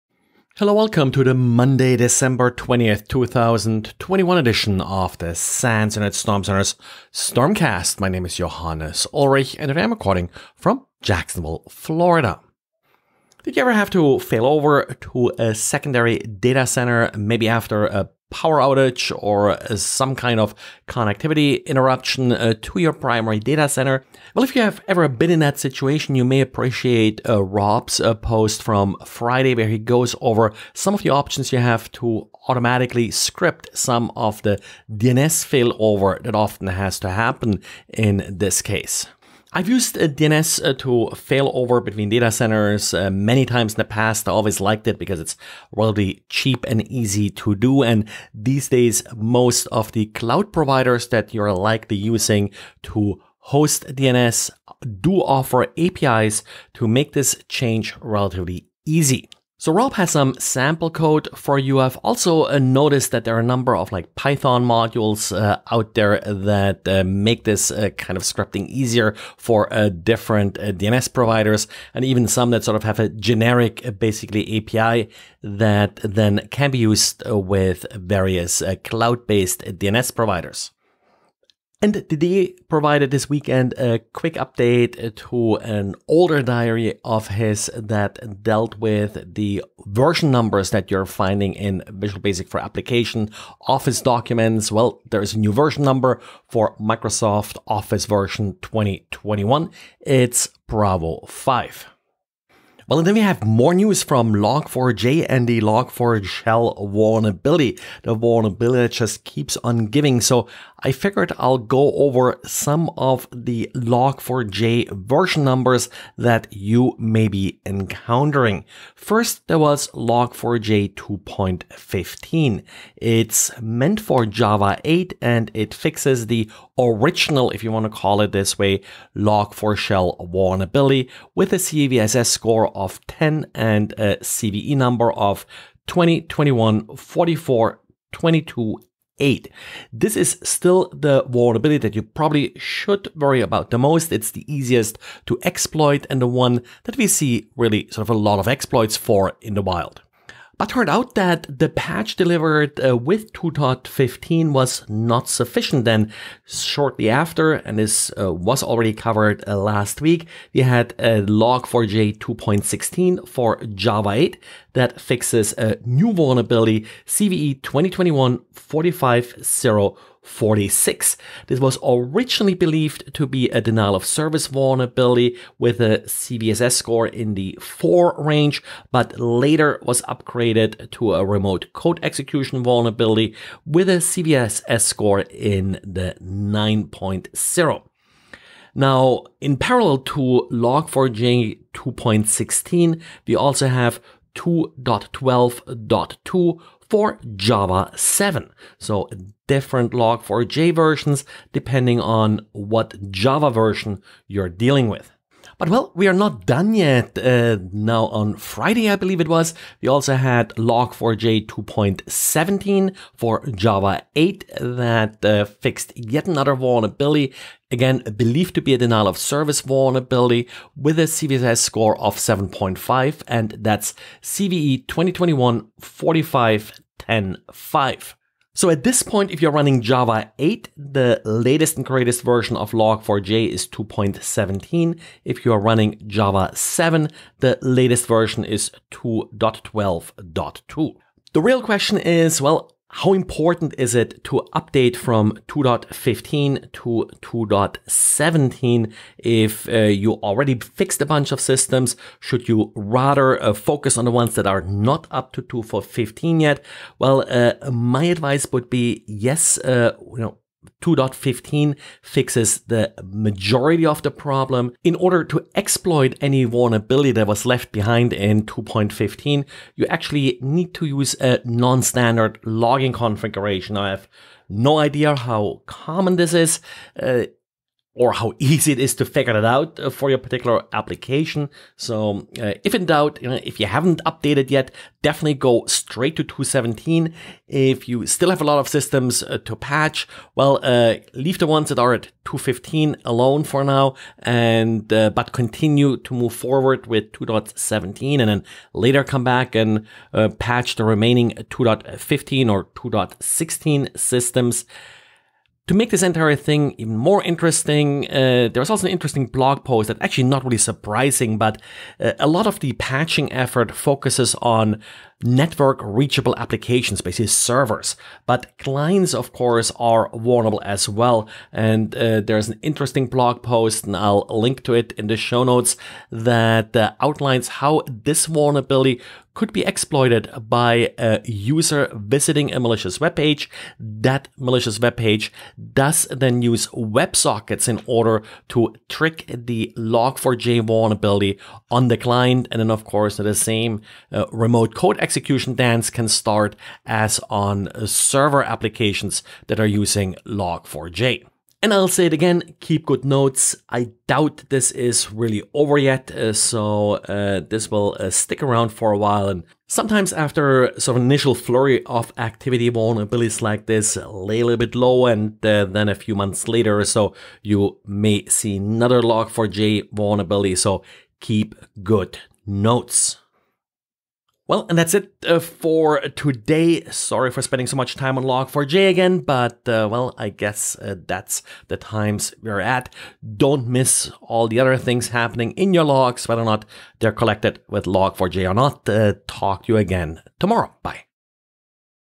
Network Security News Summary for Monday December 20th, 2021
Automating Public DNS Changes; Office 2021 VPA Version; More Log4j/Log4Shell fun